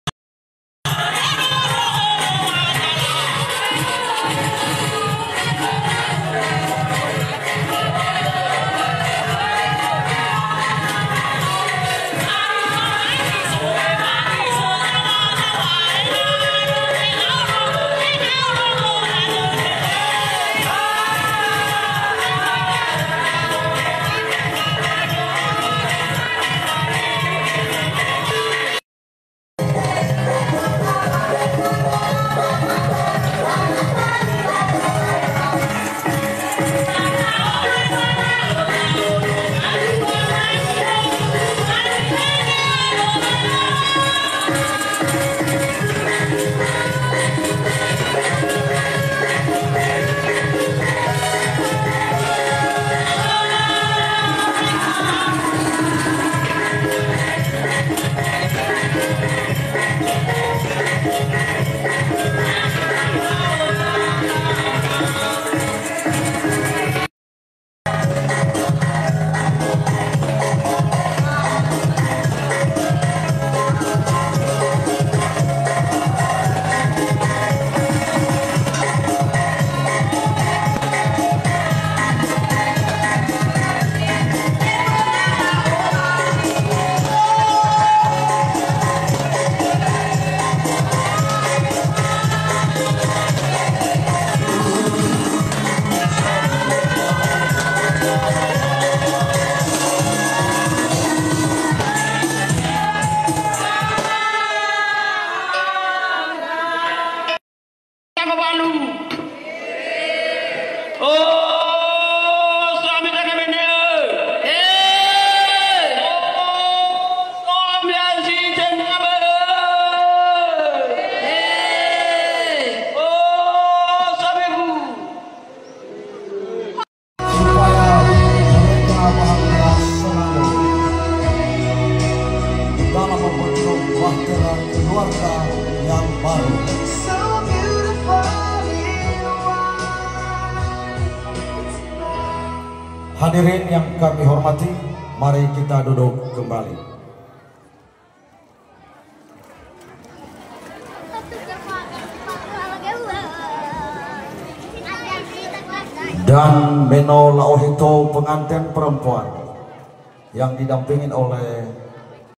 Acara Pernikahan Natalia N. I. Gulo, S.Keb. & Dominikus Lahagu, ST.
Acara Pernikahan menurut adat Nias.